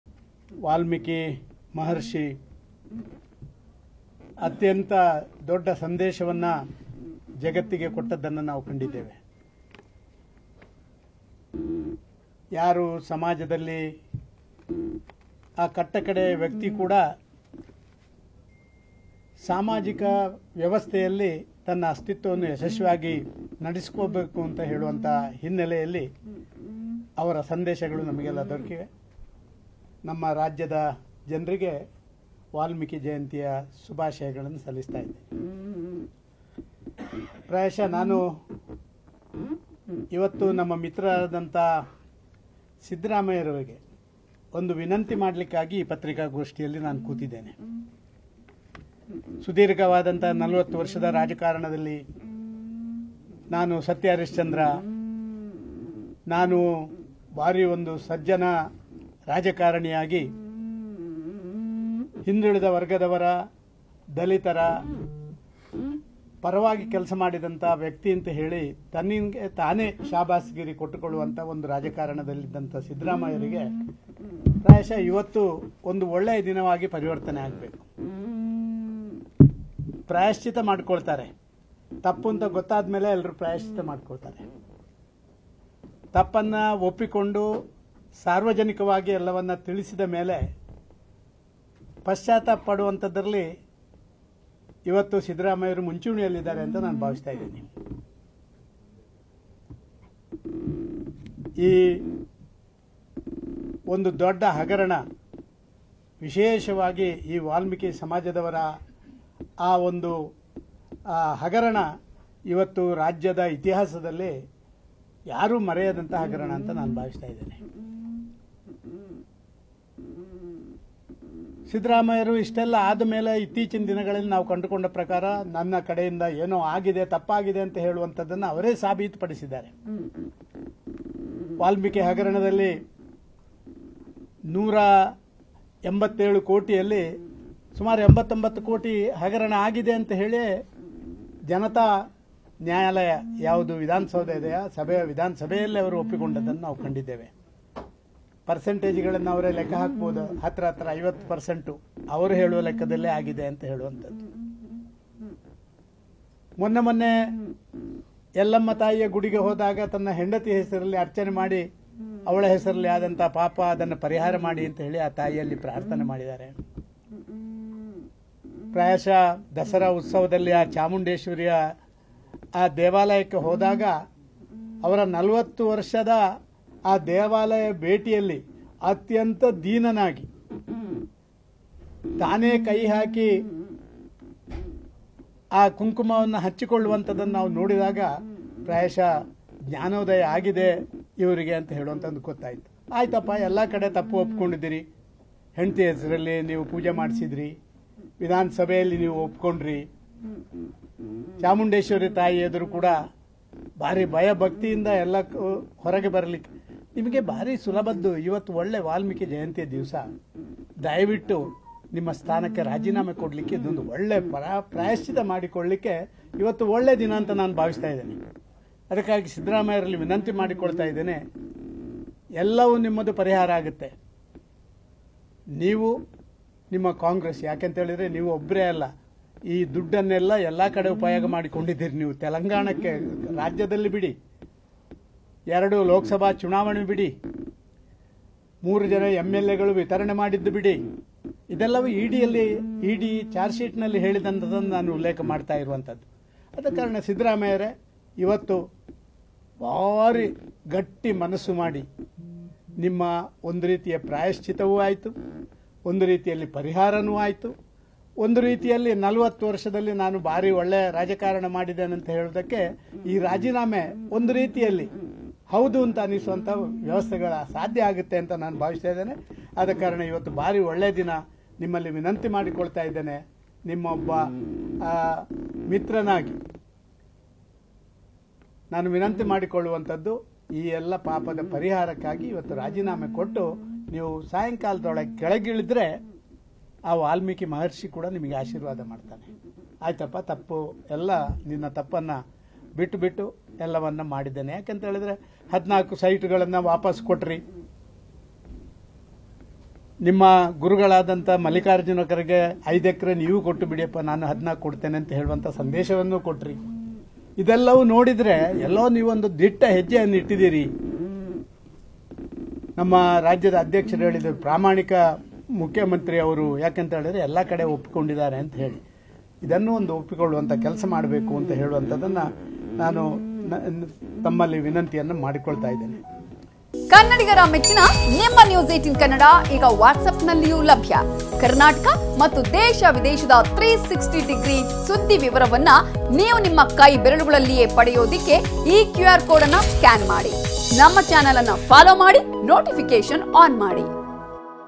DV Sadananda Gowda On CM Siddaramaih Wife | ಇಂದು ಸಿದ್ದರಾಮಯ್ಯ ರಾಜೀನಾಮೆ ಕೊಡಲು ಒಳ್ಳೆ ದಿನ | N18V
News18 Kannada Live | ನ್ಯೂಸ್ 18 ಕನ್ನಡ ಲೈವ್
Karnataka | ಕರ್ನಾಟಕ
Kannada | ಕನ್ನಡ